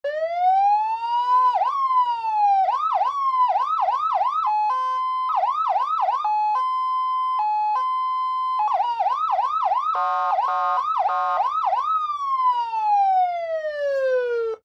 Twin Engine Fire Boat Siren Wailing with Several Variations
Twin Engine Fire Boat Siren Wailing with Several Variations · Sound Ideas

Sirens